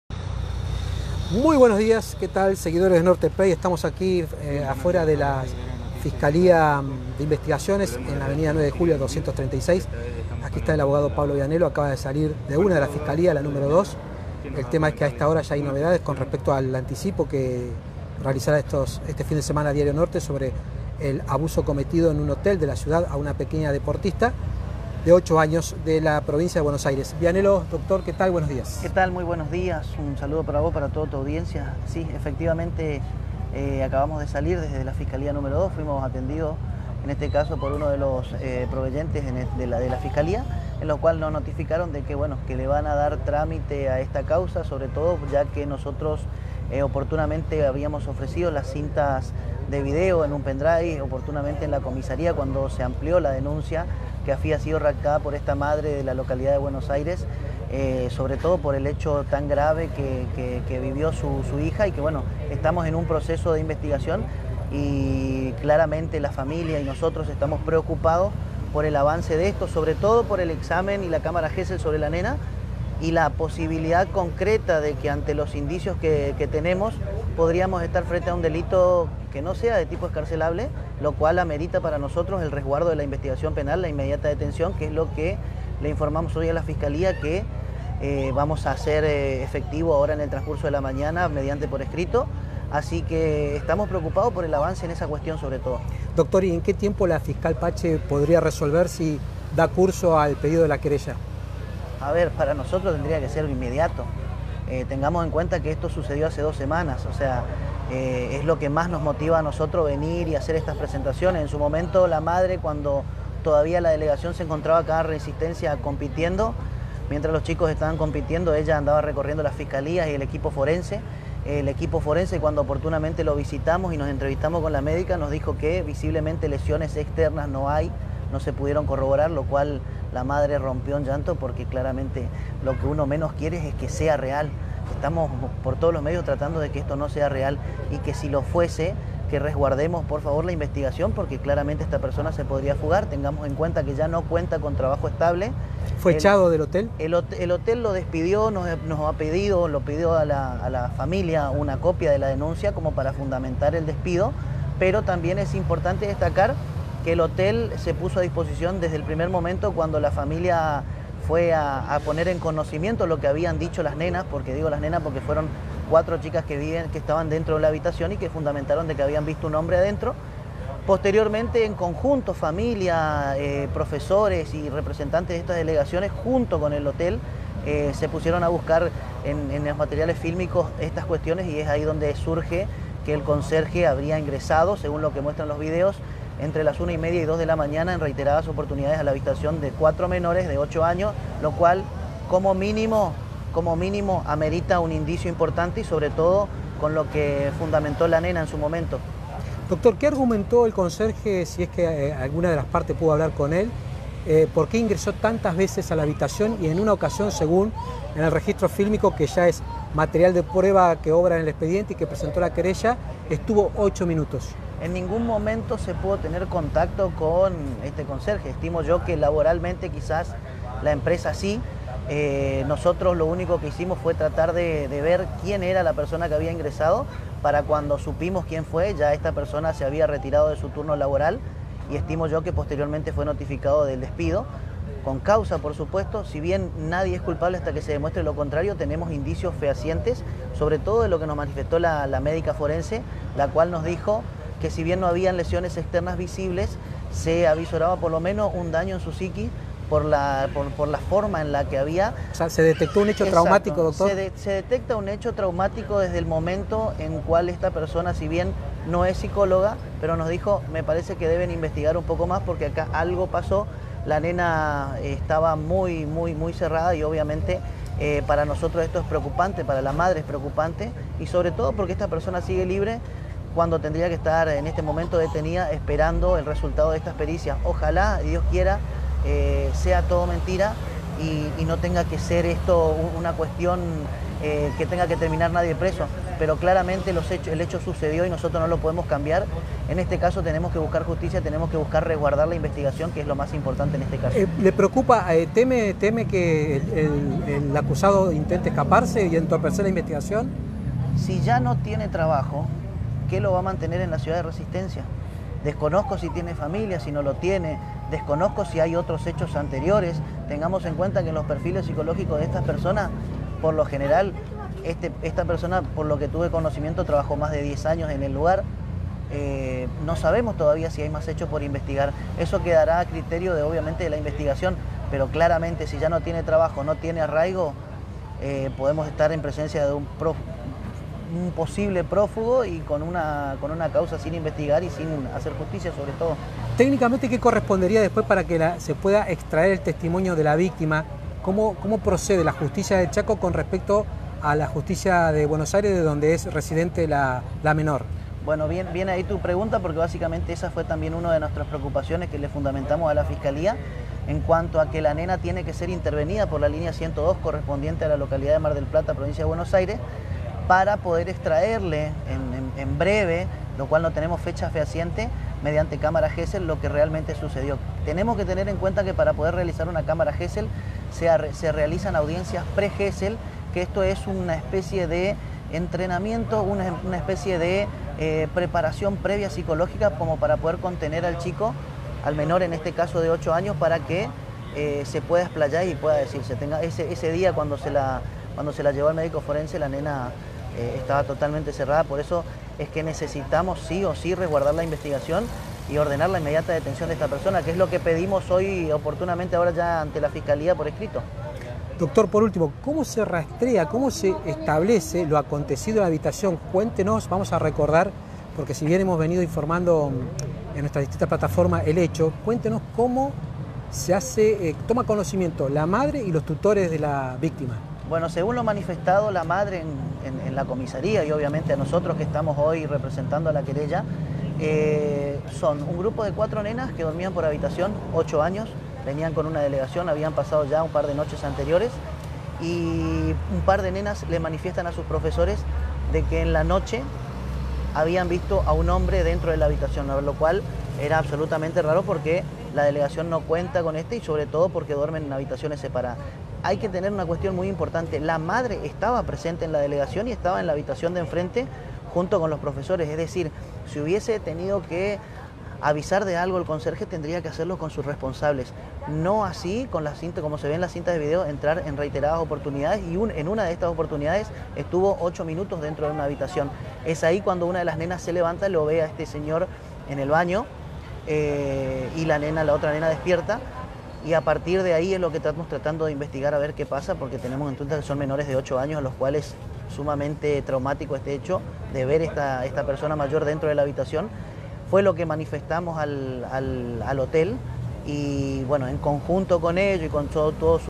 Abuso menor en hotel de Resistencia: piden la detención del conserje
Así lo señaló a NORTE el abogado Pablo Vianello desde la fiscalía de investigaciones N⁰2 de Ana González de Pacce.